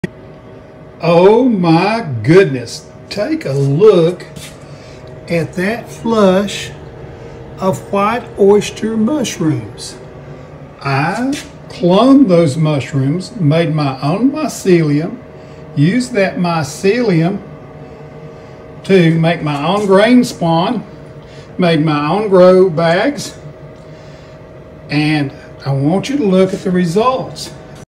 Martha Tent Update
- Just a short update on my Martha Tent and the White Oyster Mushrooms I cloned. Enjoy!